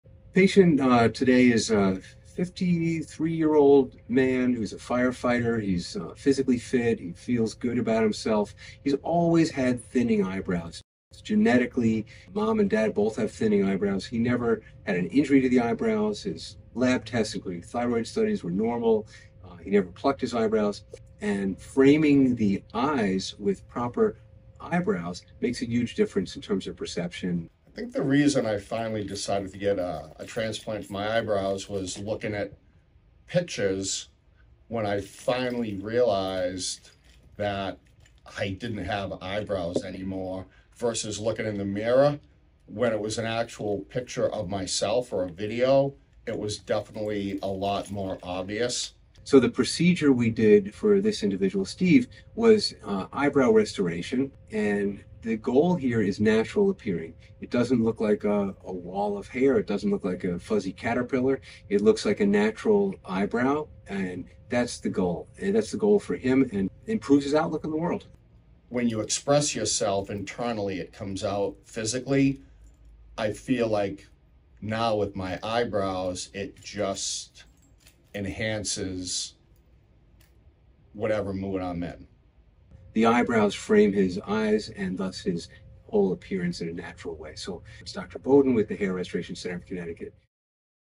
Firefighter Has His Eyebrows Restored
This patient is a 53-year-old firefighter who always had thin eyebrows, which became more evident over time. He notes how important eyebrows are in properly drawing attention to the eyes. He was self-conscious that his eyes had lost their framing. Dr. Boden performed a small FUT procedure to transplant more vigorous hair.